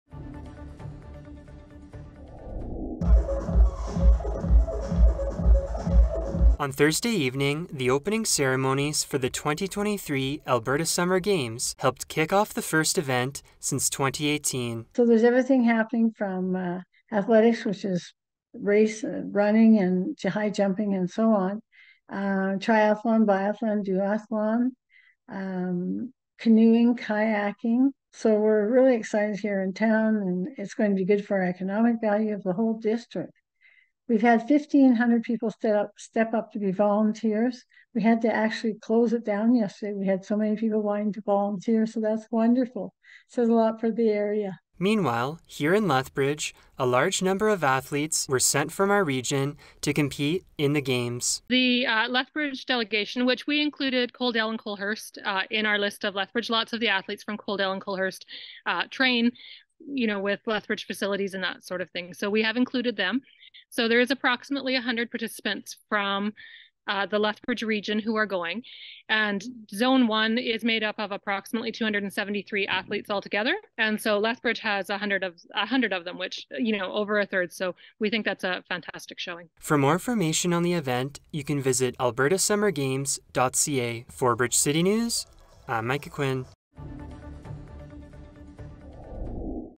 The 2023 Alberta Summer Games Underway | Friday, July 21, 2023 | Micah Quinn | Bridge City News
The 2023 Alberta Summer Games are underway with over 3800 youth athletes, coaches and managers coming together from across the province to compete.  A delegation from Lethbridge has also been sent to compete at the games taking place in Okotoks and Black Diamond until July 23rd. Video Journalist Micah Quinn has the details on the first games to be held since the start of the COVID-19 pandemic.

Aired on: Friday, July 21, 2023